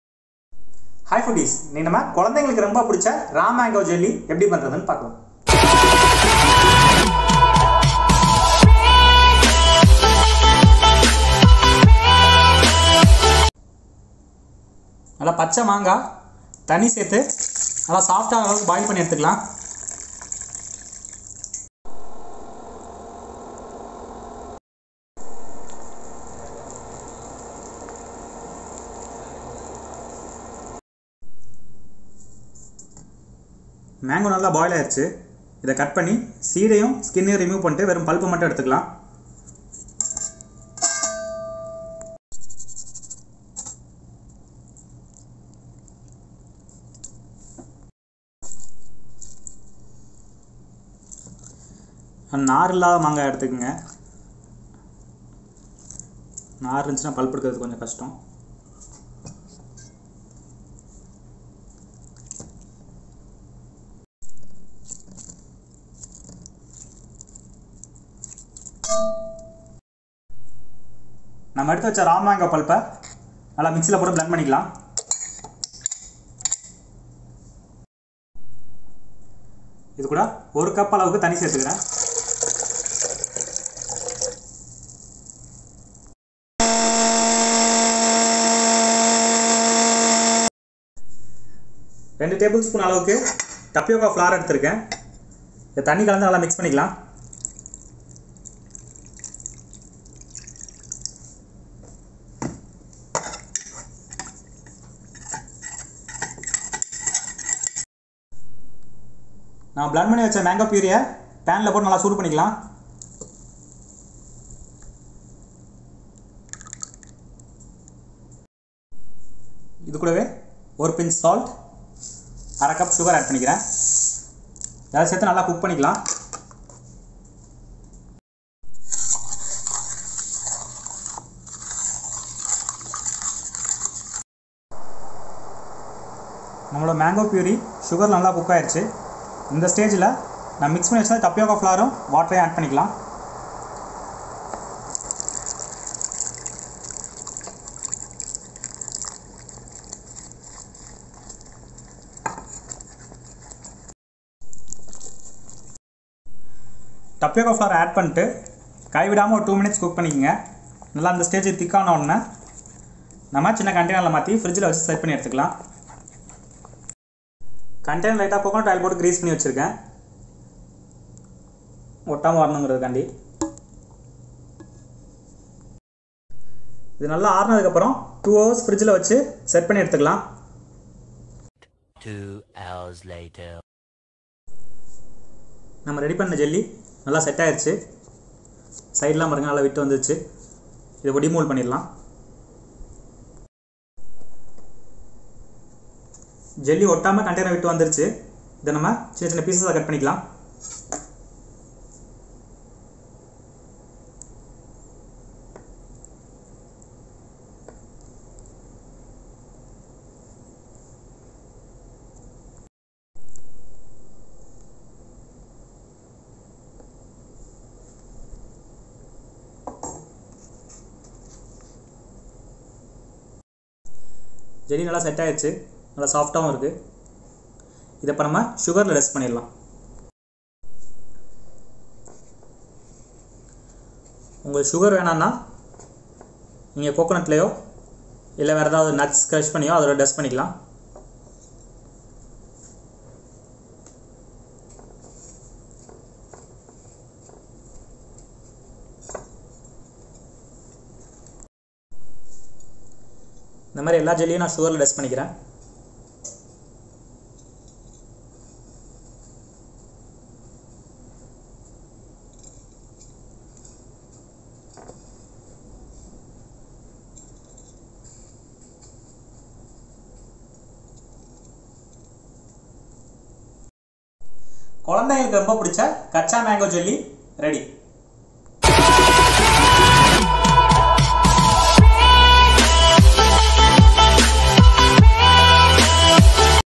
வீட்டிலேயே செய்யலாம் ஈஸியான ஜெல்லி / raw mango jelly
How to make delicious raw mango jelly by using easily available simple ingredients
Wanna try the same!
Follow below quantities for better quality & taste.